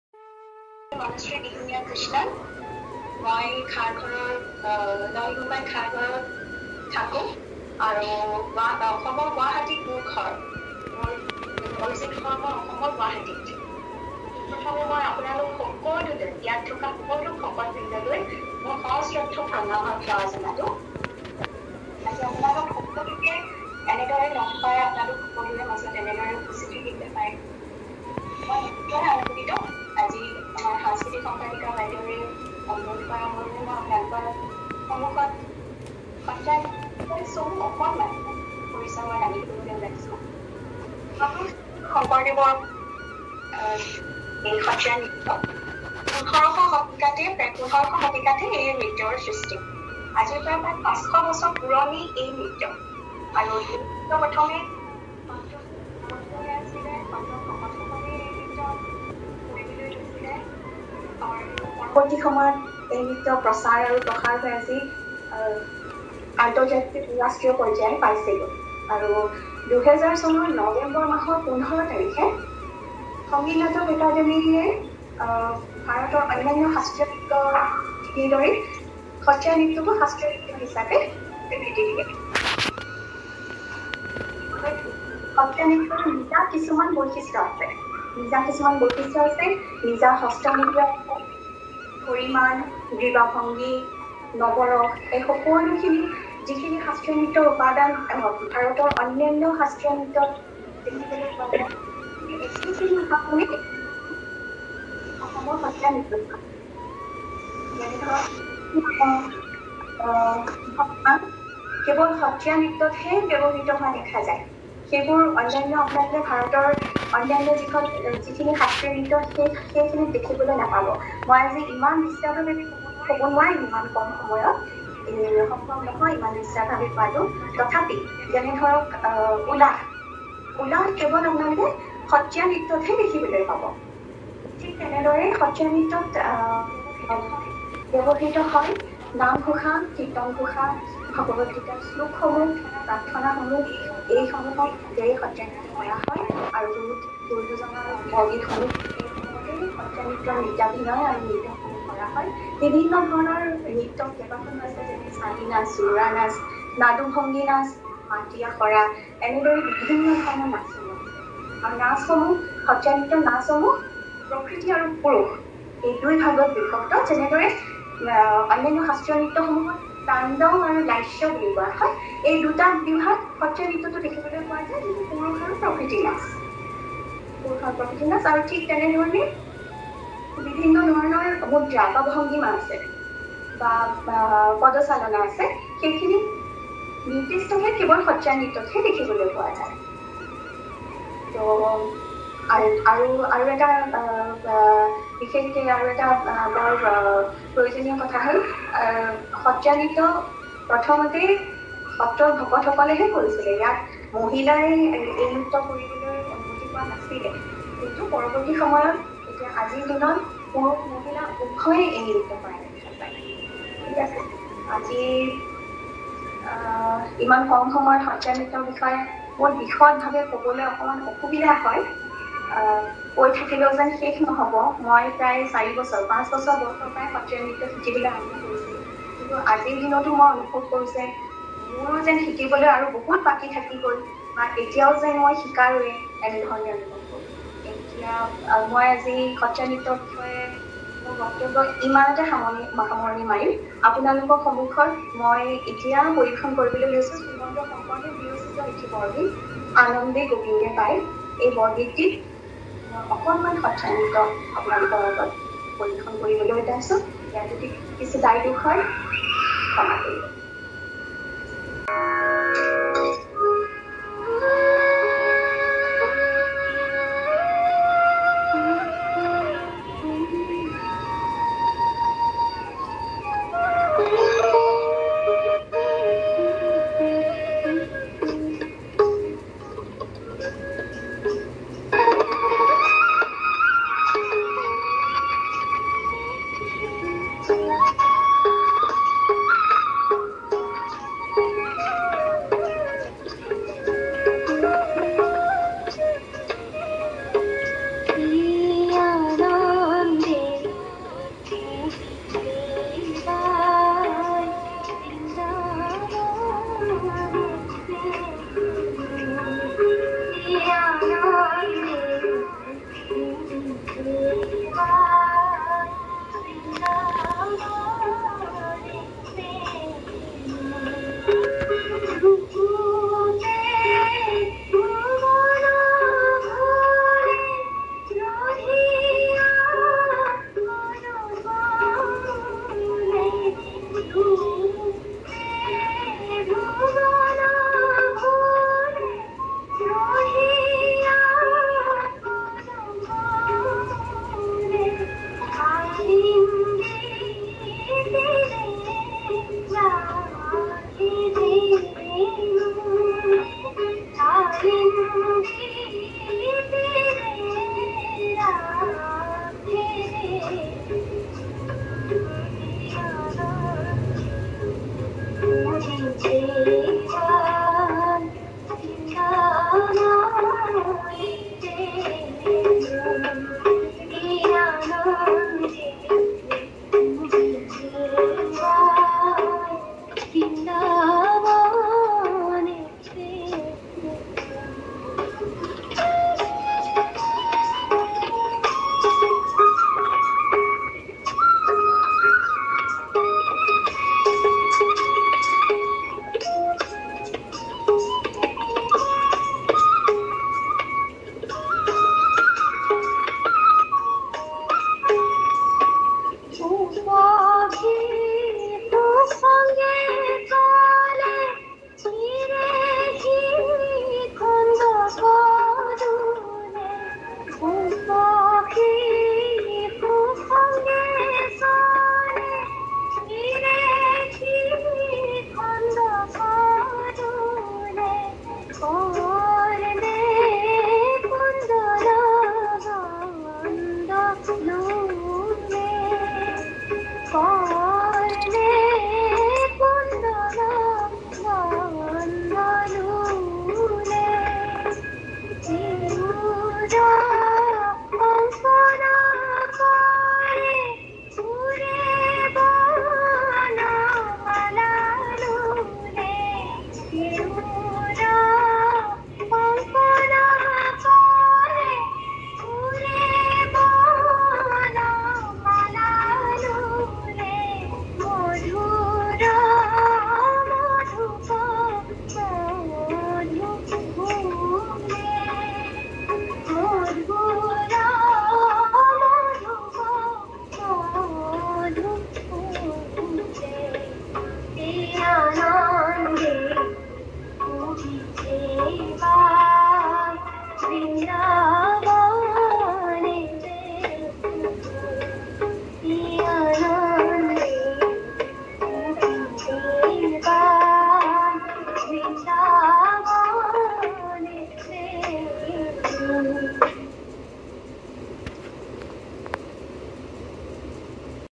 Hari_Naam : Week 1 Learn Interesting facts about Satriya Dance : Classical Dance from Assam
Hari_Naam : Week 1
Satriya Dance - Indian Classical Dance from the Land of Assam.

Know interesting facts about Satriya Dance, this beautiful classical dance as explained by Ms. Manashri Dihingia Kanan ji

Join us in our weekly Spiritual event every Saturday 6 PM to 7 PM IST